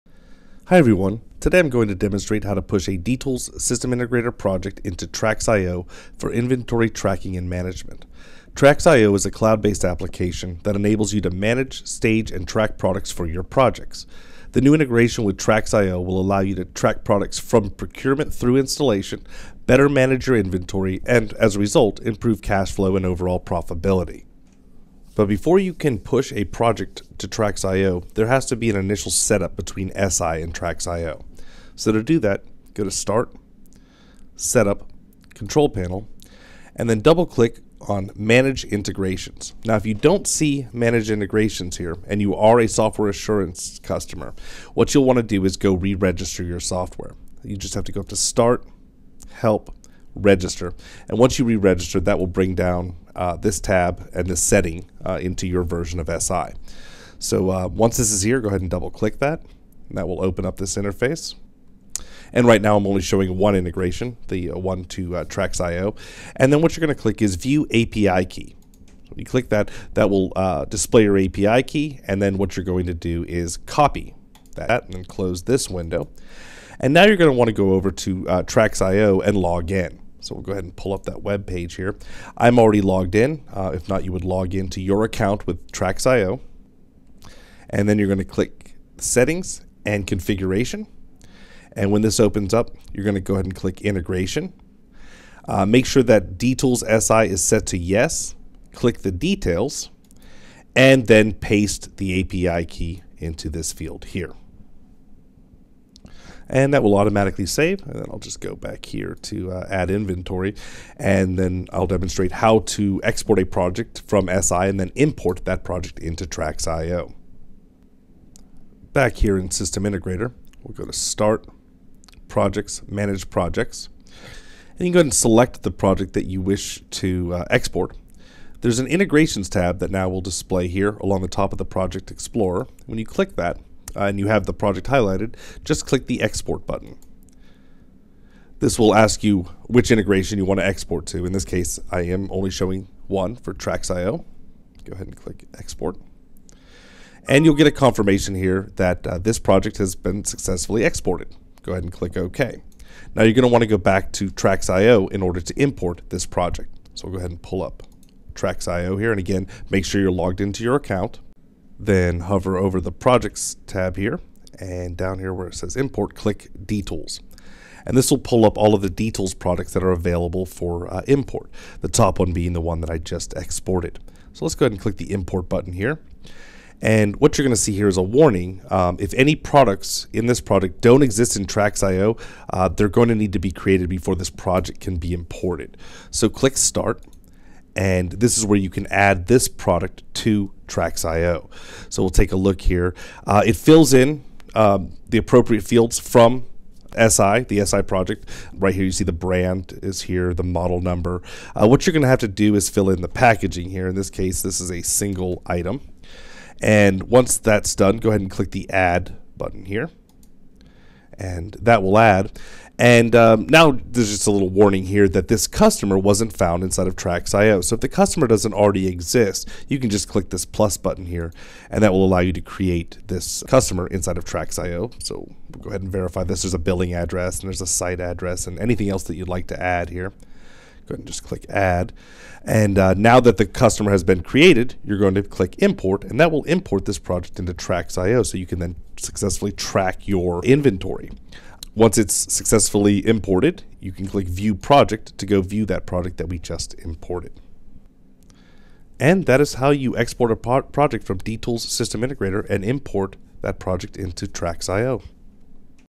D-Tools SI and TRXio Integration
This video will demonstrate exporting a D-Tools SI Project and importing that project into TRXio.